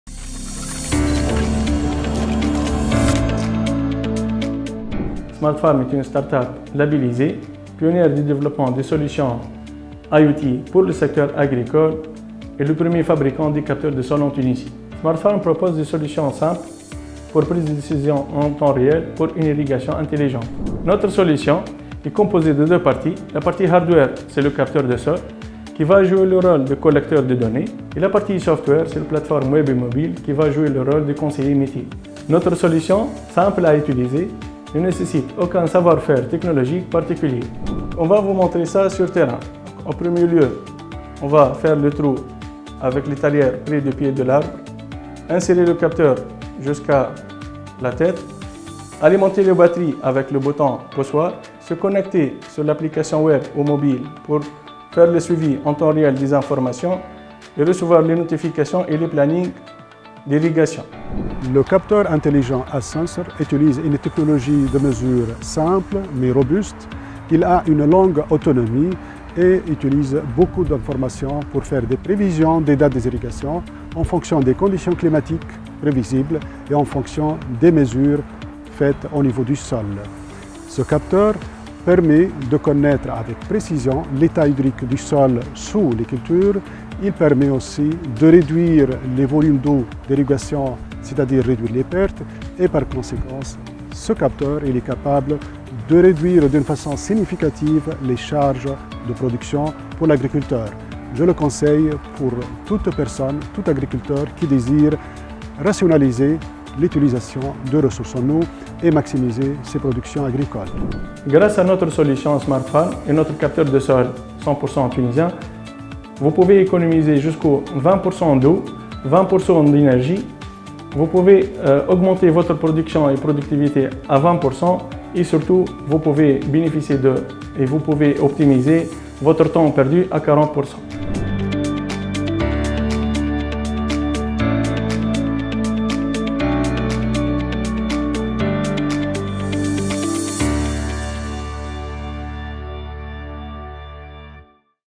Smart Soft Pro: Smart Farm la solution d'irrigation intelligente
Smart Farm est une solution d'irrigation intelligente qui permet d'optmiser la consommation d'eau d'irrigation en économisant 20% d'eau, tout en augmentant la production de 20% et en réduisant les frais de production de 40%.